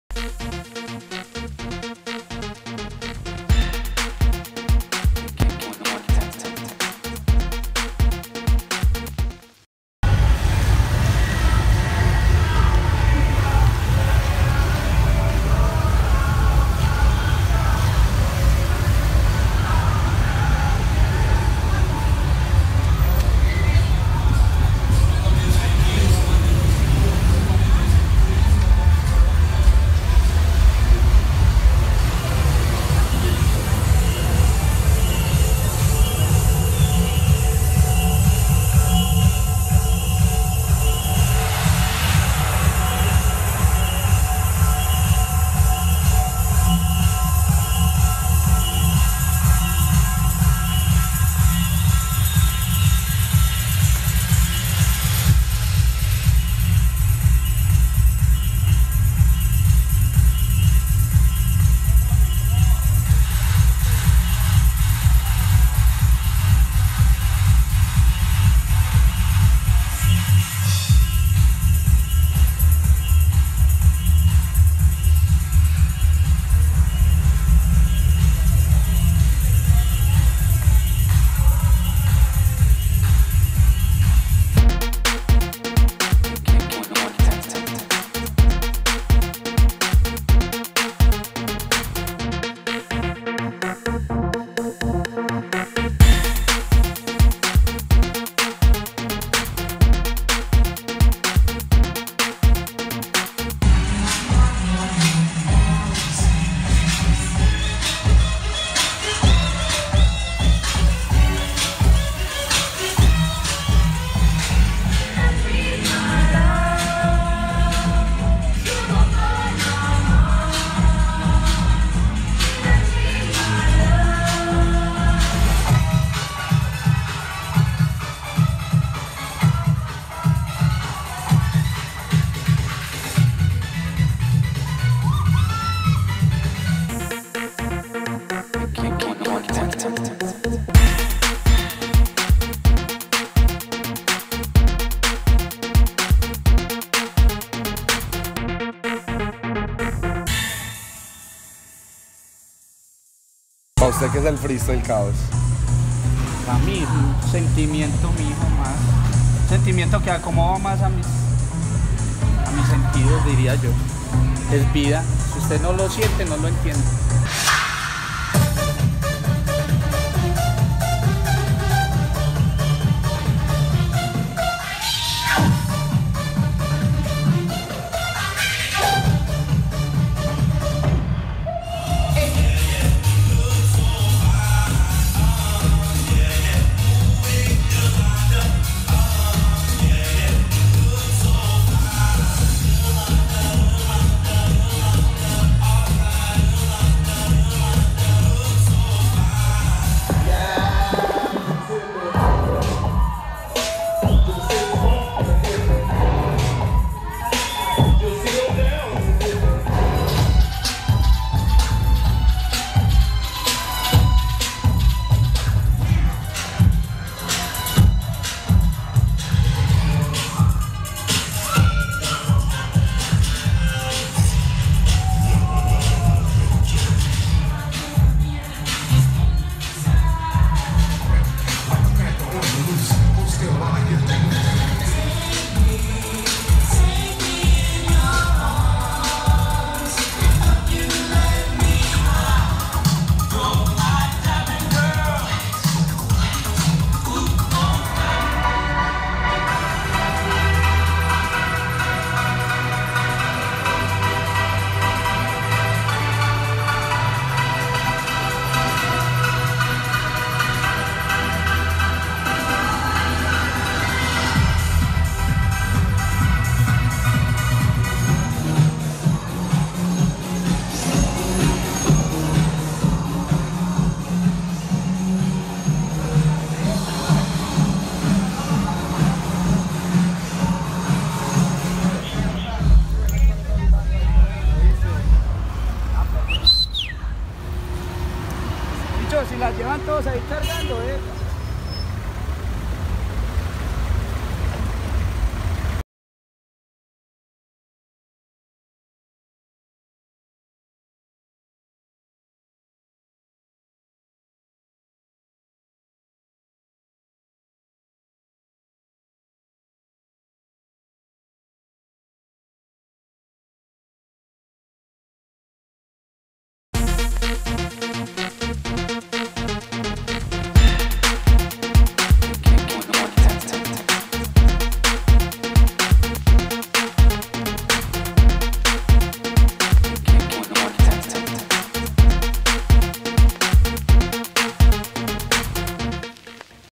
Freestyle Medellin - Capítulo 6
Caos por fin pudo ver la bola disco que tenia en mente y ante 500 personas mezclo freestyle.